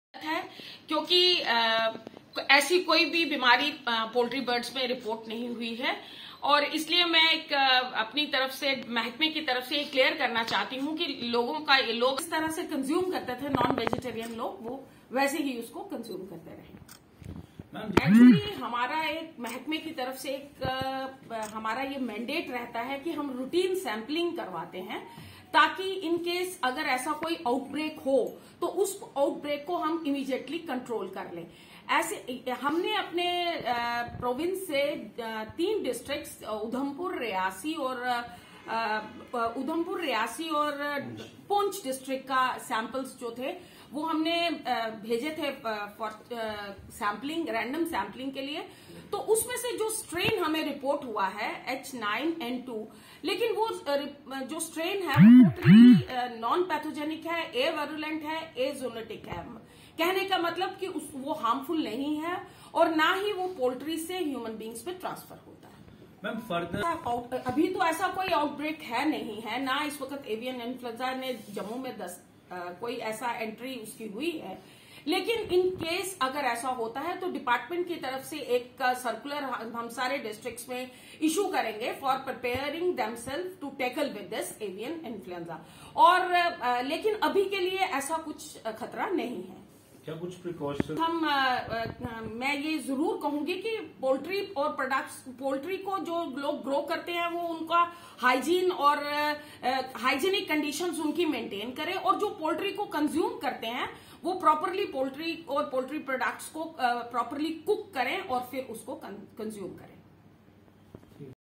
Press conferance regarding Bird Flu at Direcorate of Animal Husbandry
Press conferance regarding Bird Flu at Direcorate of Animal Husbandry Talab tillo jammu today.

Earlypost#Jammu&kashmir#Jammu#J&k#Press₹conferance#Bird#Flu₹Direcorate#Animal#Husbandry#Talab#tillo